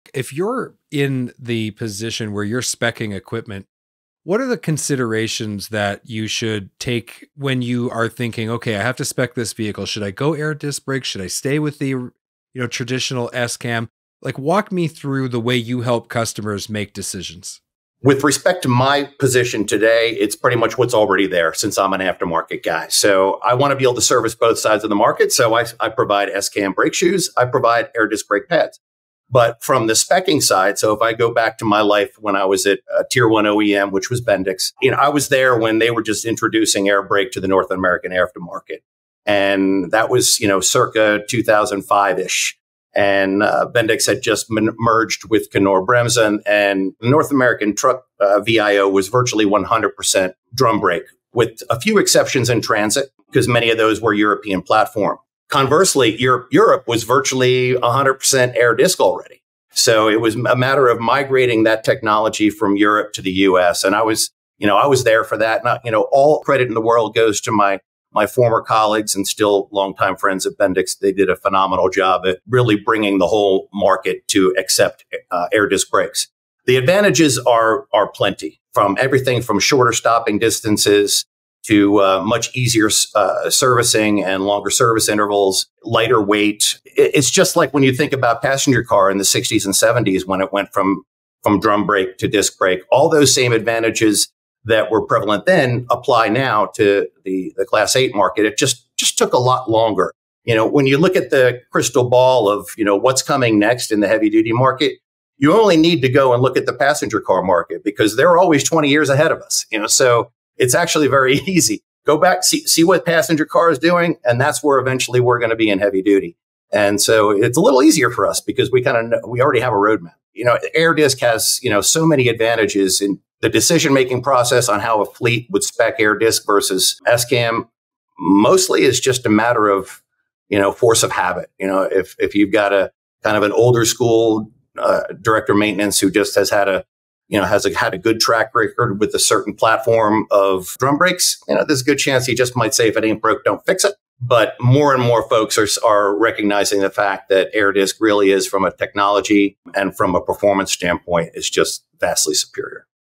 Air Disc vs. S-Cam Brakes: Making the Right Choice for Your Fleet
Spec'ing equipment in the heavy-duty trucking industry is a critical decision, affecting both safety and performance. This video compares air disc brakes with traditional S-Cam brakes, highlighting the advantages of air disc brakes, including shorter stopping distances, easier servicing, longer service intervals, and a lightweight design. While some fleets resist change due to the "if it ain't broke, don't fix it" mindset, industry experts increasingly endorse air disc brakes for their technological superiority and enhanced performance. Ultimately, the decision comes down to prioritizing safety, performance, and efficiency. Share your brake preference in the comments.

Become a Guest on the Podcast!

Advertise on the Heavy Duty Parts Report!